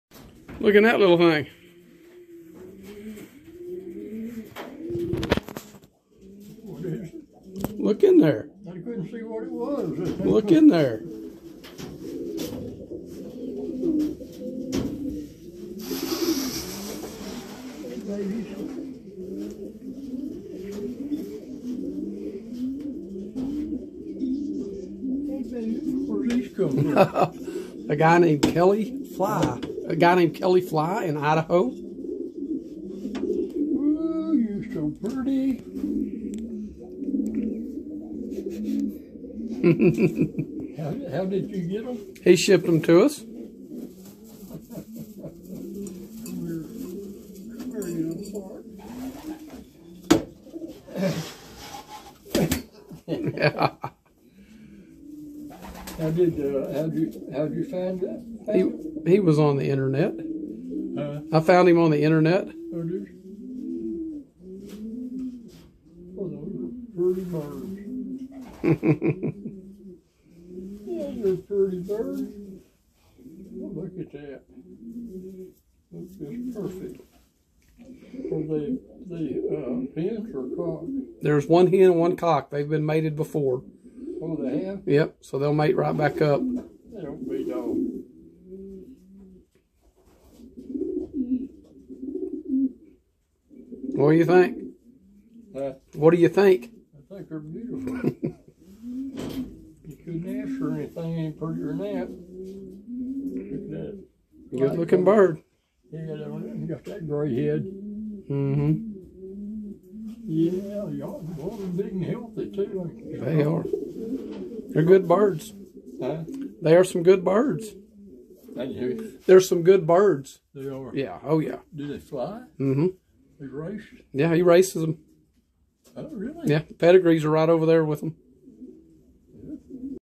Dad gets his new Yellow Racing Pigeons.
through the generosity of the pigeons fancy here in the USA we were able to find some yellow bar racing pigeons for dad to replace one that had become sick and passed away.